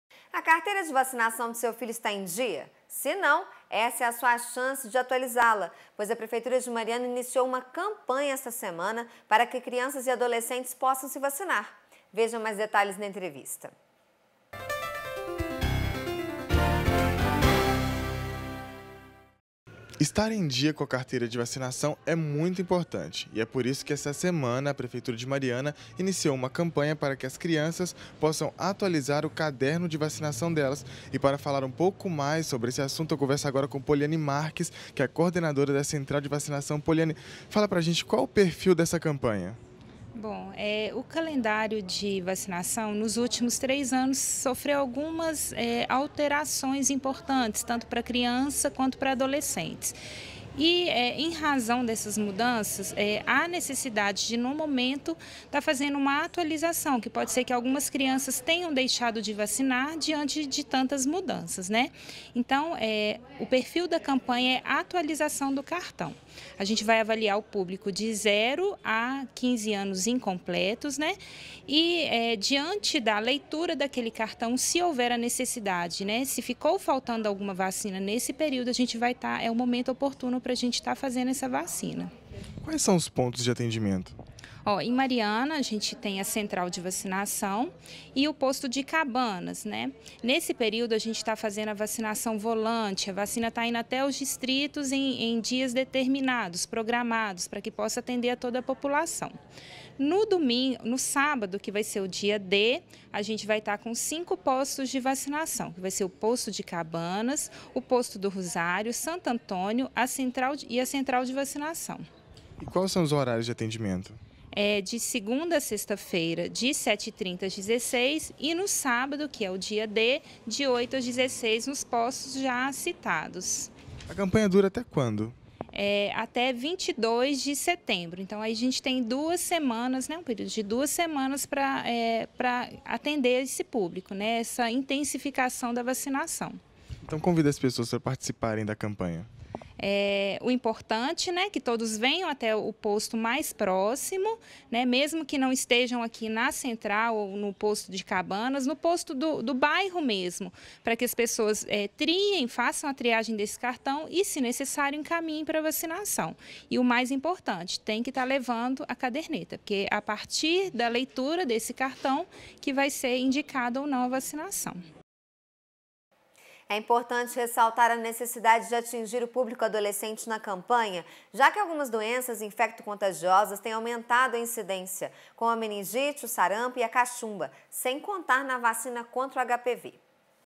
Mariana realiza campanha de atualização da caderneta de vacina para crianças e adolescentes
A carteira de vacinação do seu filho está em dia? Se não, essa é a sua chance de atualizá-la, pois a prefeitura de Mariana iniciou uma campanha esta semana para que crianças e adolescentes possam se vacinar. Veja mais detalhes na entrevista.